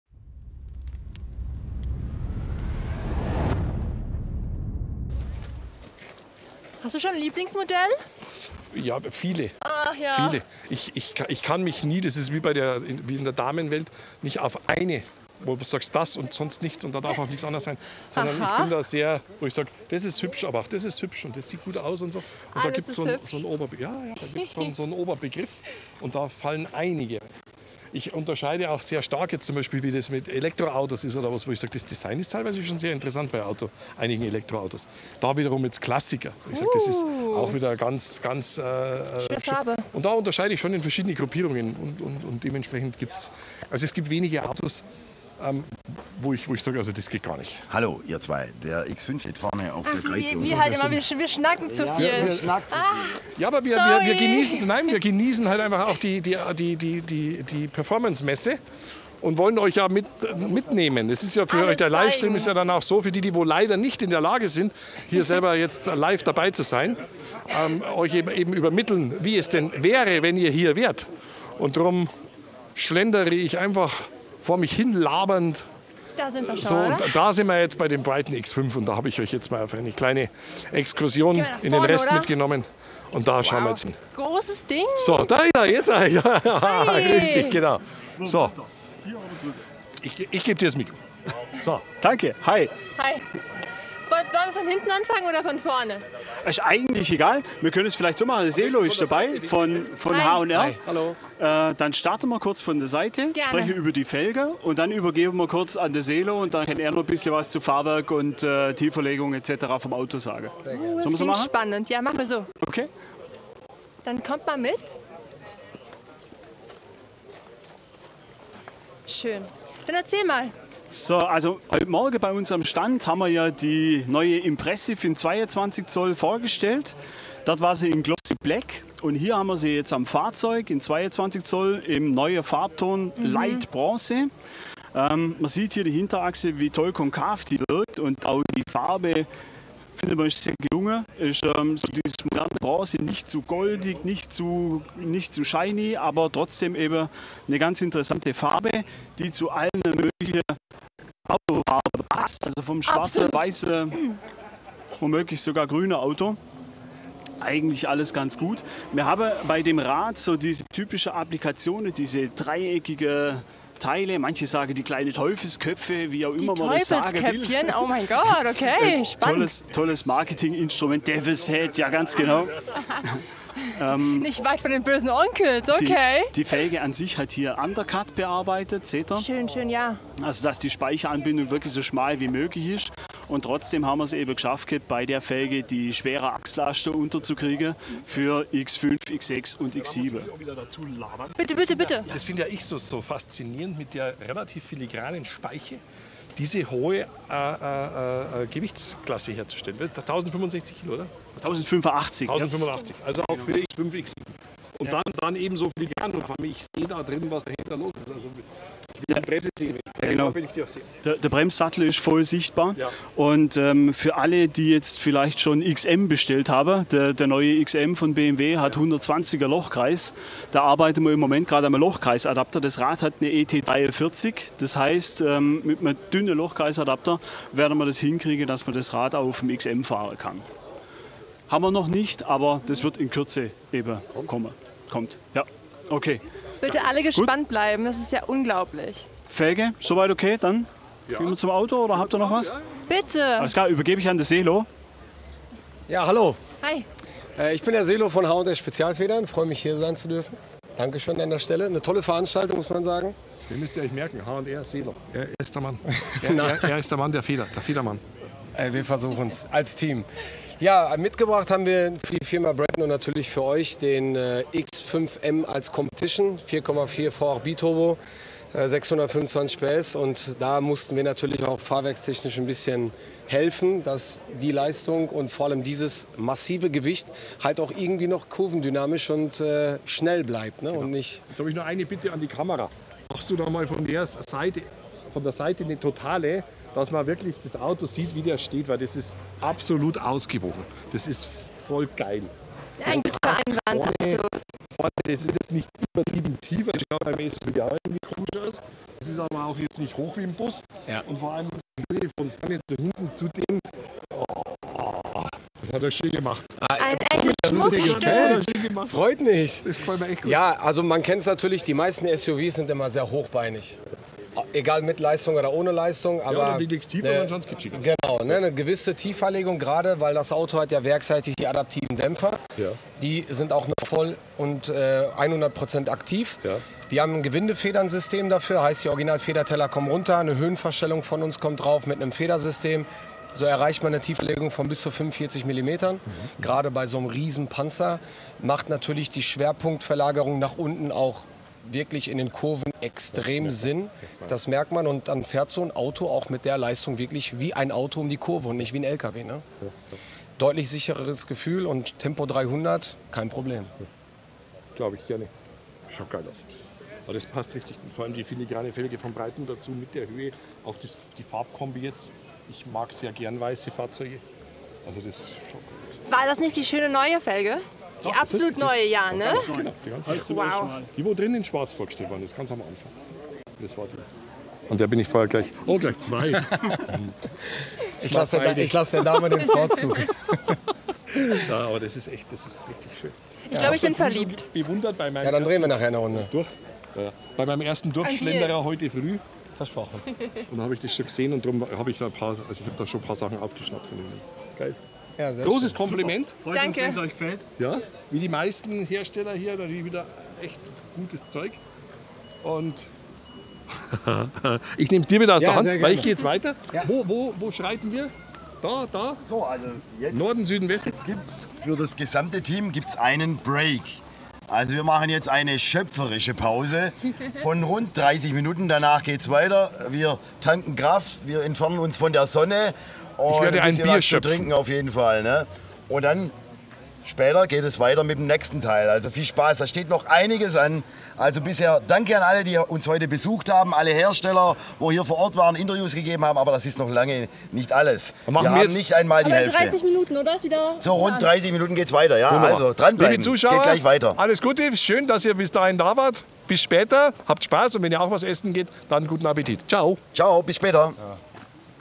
&
Breyton stellte auf der eine neue Felge. Hier montiert auf dem X5 M die Impression Felgen in 22 Zoll und der Farbe bronze. Fahrzeuge mit einer @hrspezialfedern Tieferlegungsfeder ausgestattet.
Here the Impression rims in 22 inches and the color bronze are mounted on the X5 M. Vehicles equipped with a @hrspezialfedern lowering spring.